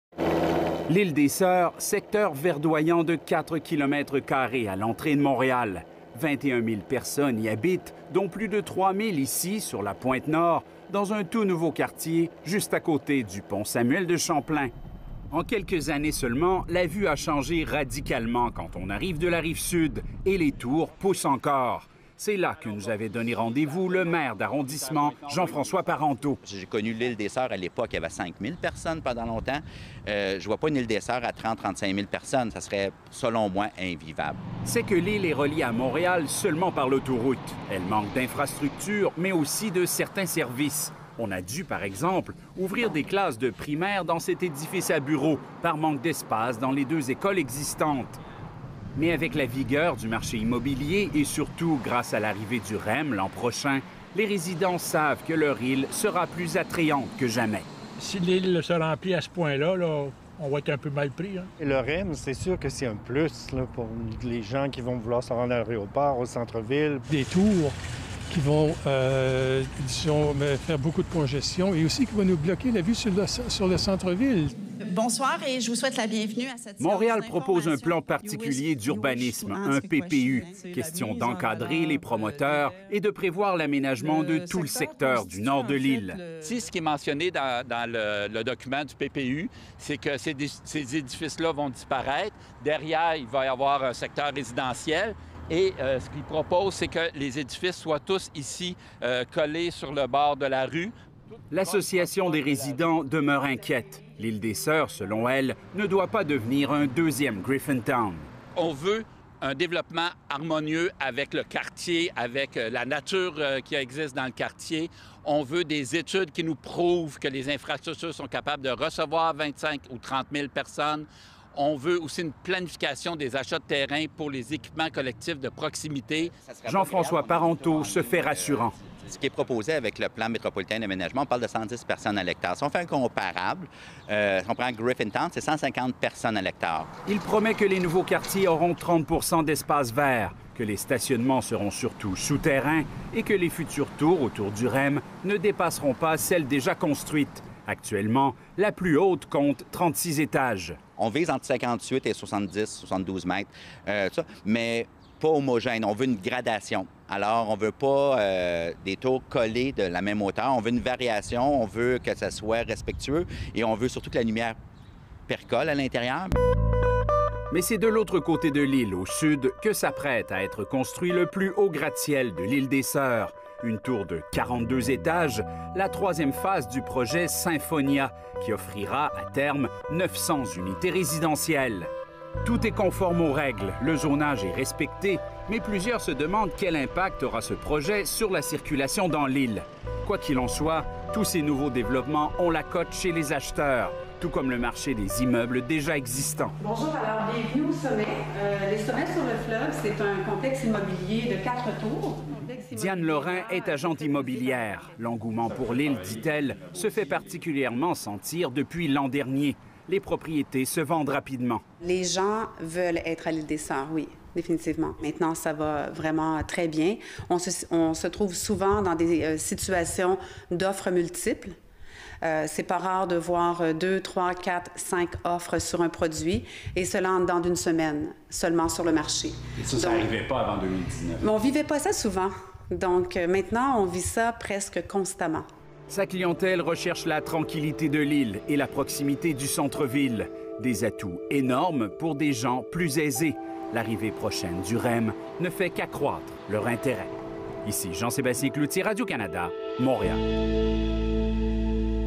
Une île transformée par le boom immobilier
C'est un phénomène répandu dans la grande région de Montréal : des quartiers qui se densifient avec la construction de tours à condo. C'est le cas à L'Île-des-Soeurs, avec l'arrivée du REM. Certains résidents ne partagent pas cet engouement. Le reportage de Jean-Sébastien Cloutier